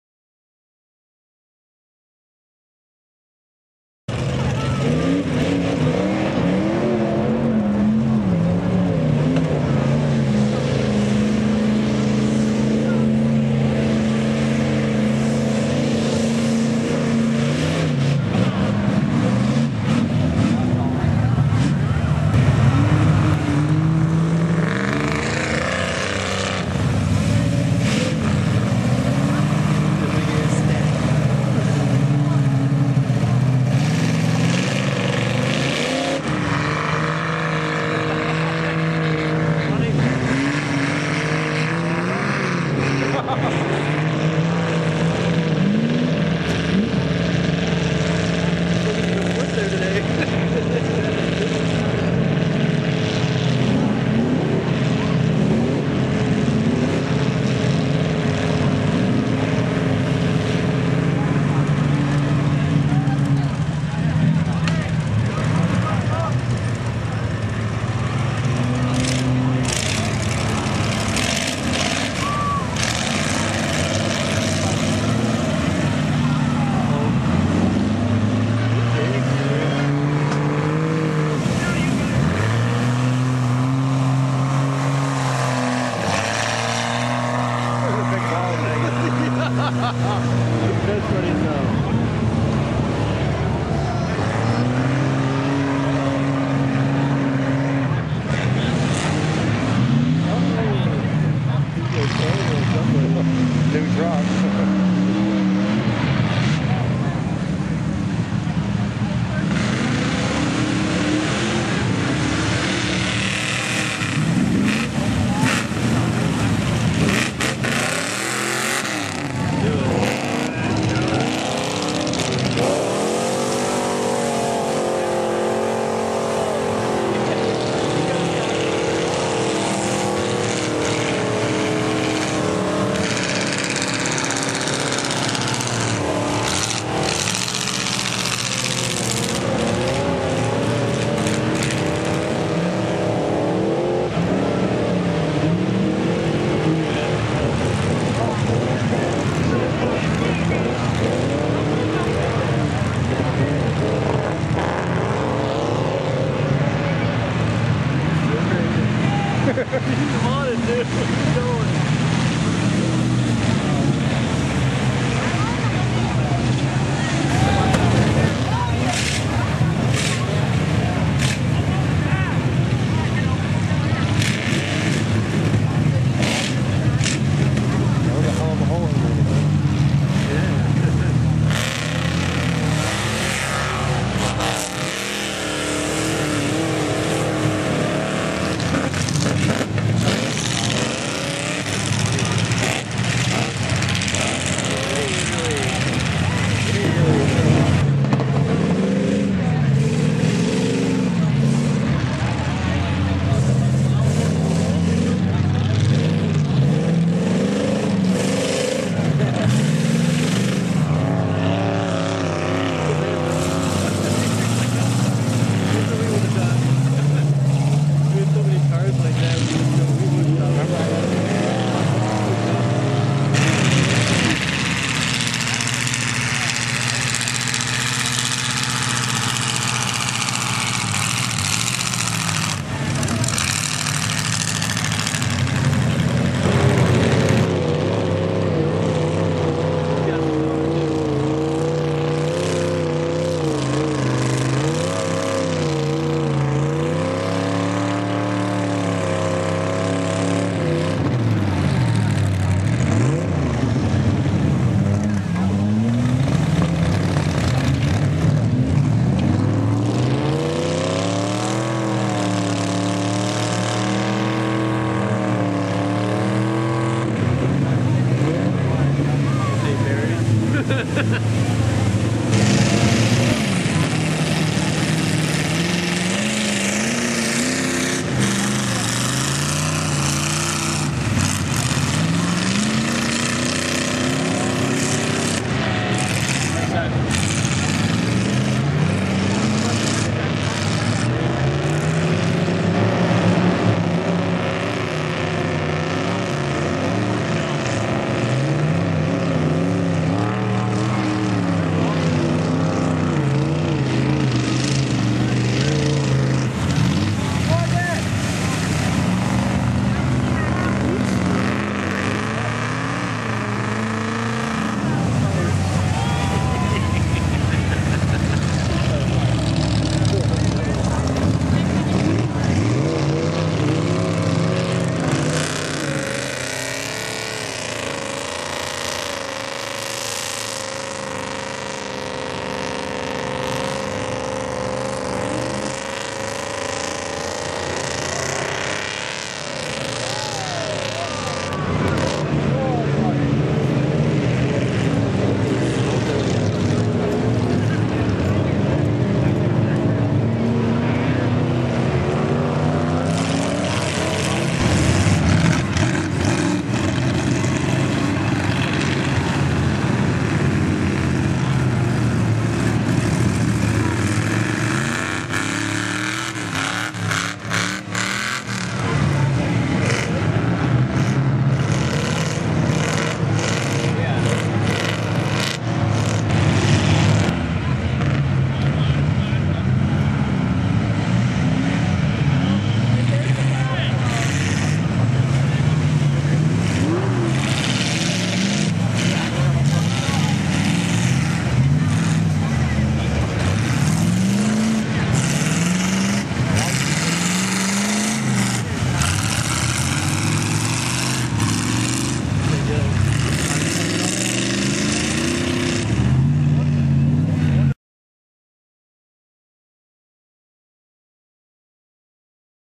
mega monster dodge truck takes on mud hole
dodge mega mud truck walks right through one of the deep holes at kirbys kompound.